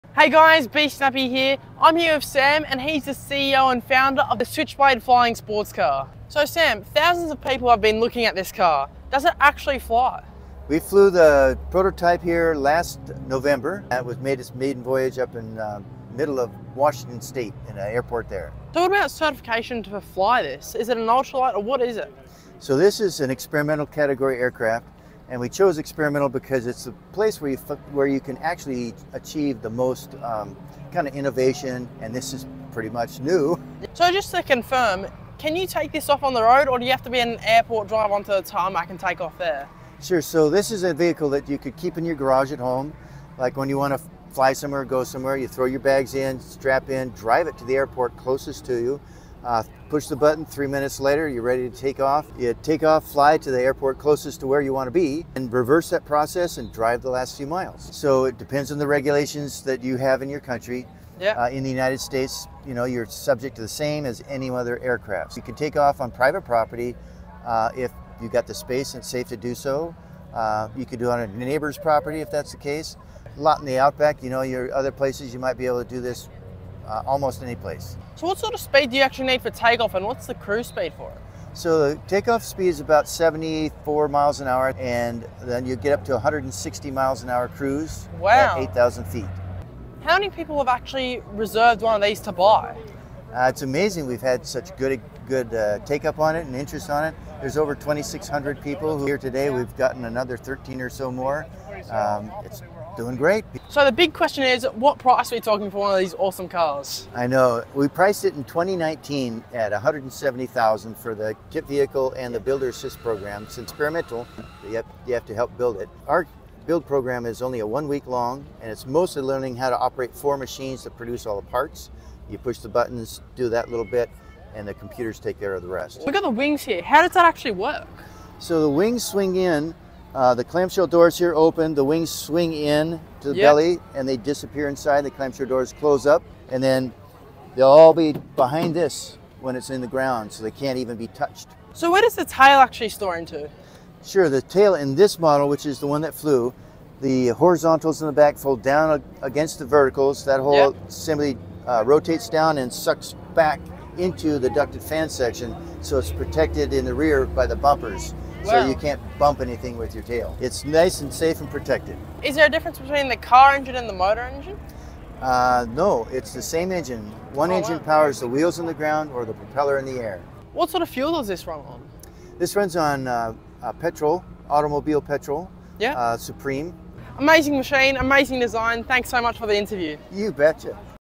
The Switchblade REAL Flying Car. Price & How the Wings Fold In.
CEO of the Switchblade flying car chats with me at Oshkosh 2024. We talk about price, certification, how the wings and tail fold in, take-off speed, cruise speed and how many expressions of interest there are.
Thank you to CEO and found a Sam Bousfield for taking the time to speak with me. Enjoy the video.